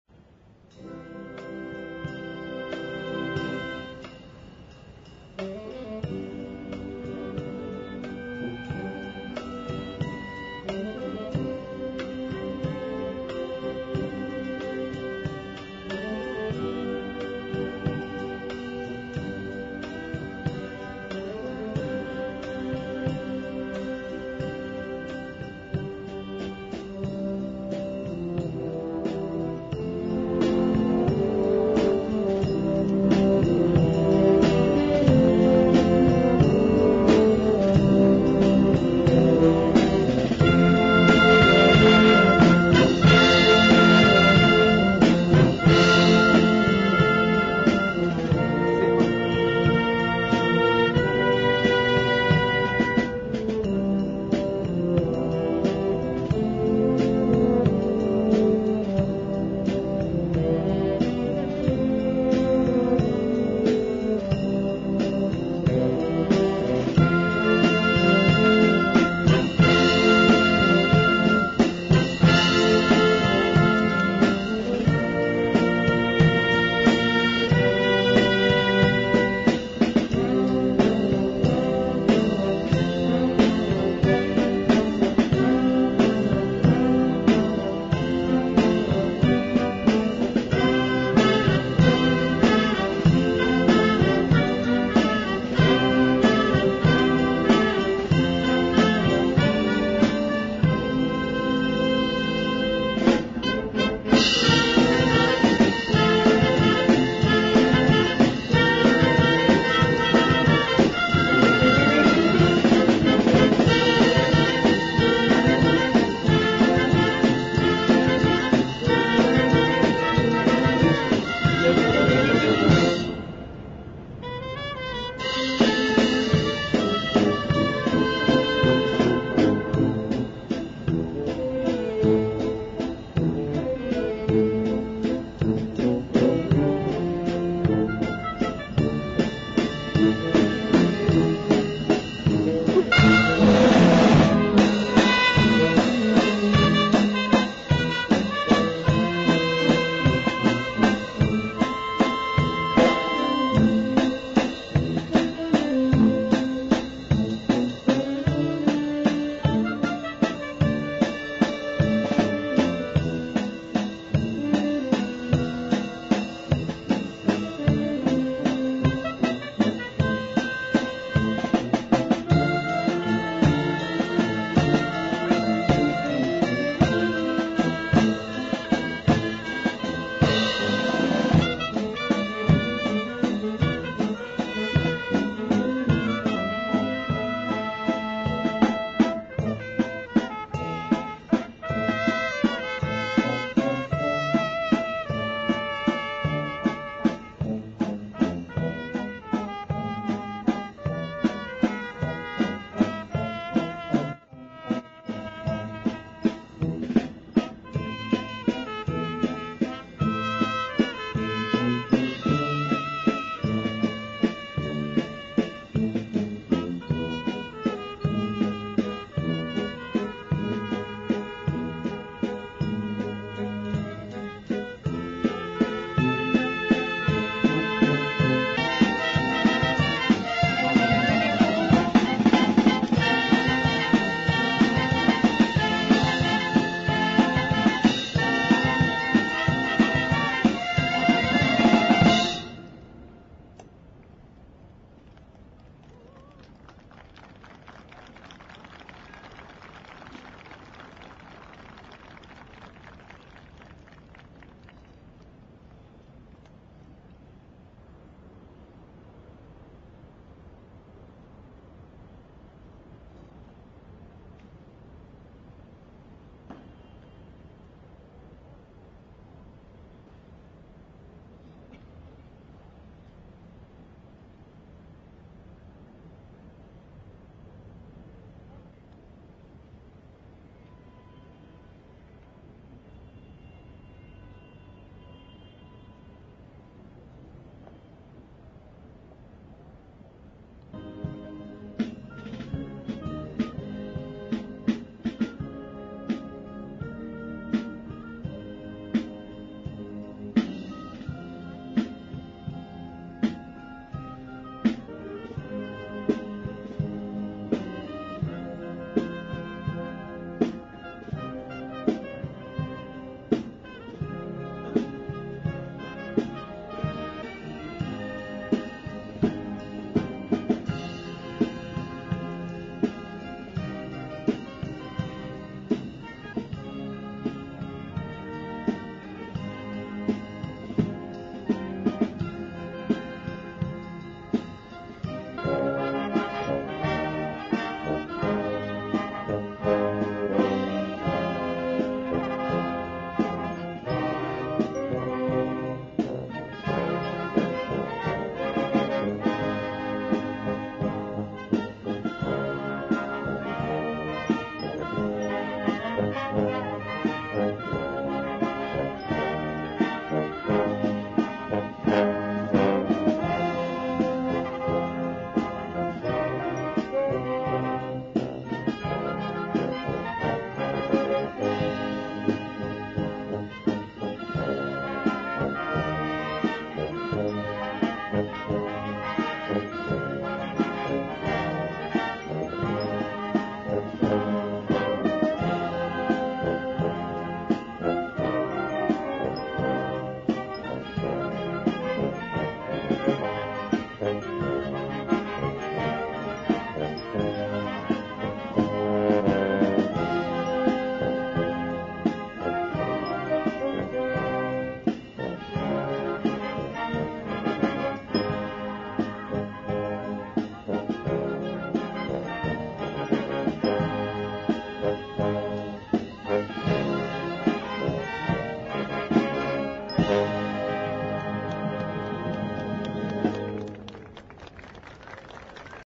Guardia de Palacio rindió tributo a Juan Gabriel frente a La Moneda
Los músicos de Carabineros tocaron hits del mexicano.